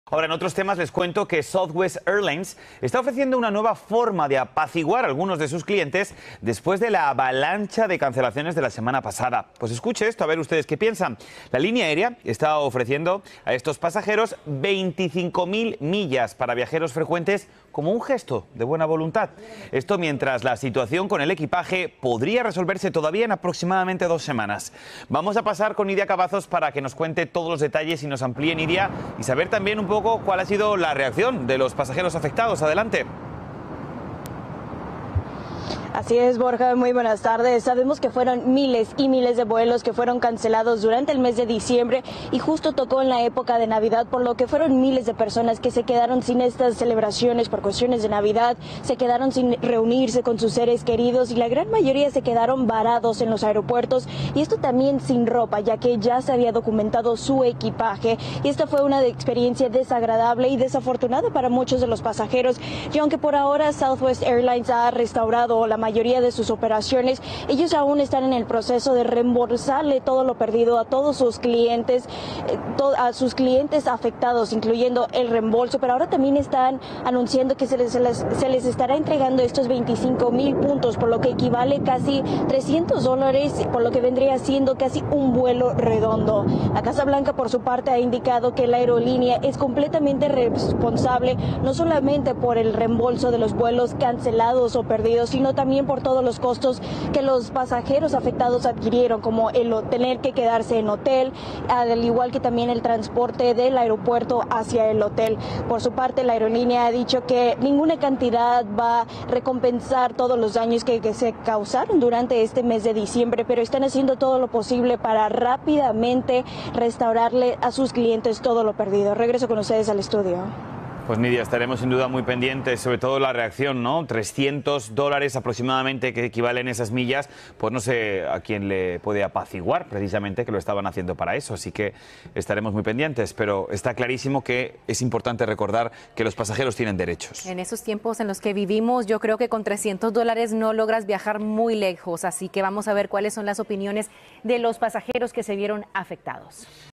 Southwest ofrece paquetes de millas para compensar a viajeros afectados por las cancelaciones
Debido a que muchos de los afectados tuvieron que pasar las celebraciones de diciembre estancados en un aeropuerto, Southwest decidió ofrecer paquetes de millas para compensar dicha situación. A la par de esto, el gobierno dejó claro que la aerolínea deberá responder no solo por los reembolsos, sino también por los gastos de hotel y transporte en los que incurrieron los viajeros por las cancelaciones.

León Krauze